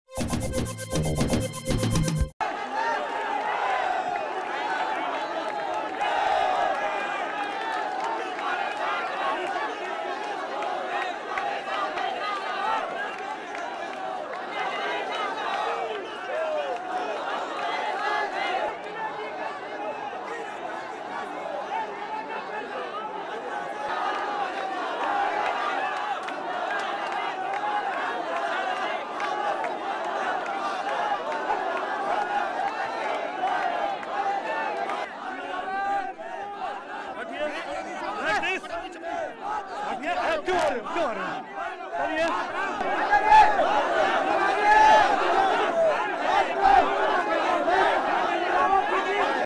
India's Anna Hazare pays respects at Gandhi memorial
Activist Anna Hazare vows a "revolution" in India as he begins a public fast amid scenes of celebratory protest that send an intimidating message to a shell-shocked government. Duration: 00:49.